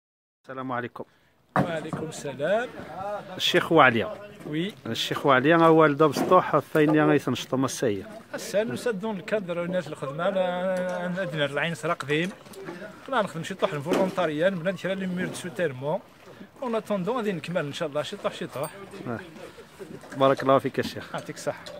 Chikh Belhadi Ouali
Volontariat pour construire des locaux aux besoins du village el ghaba cne smaoun Bejaia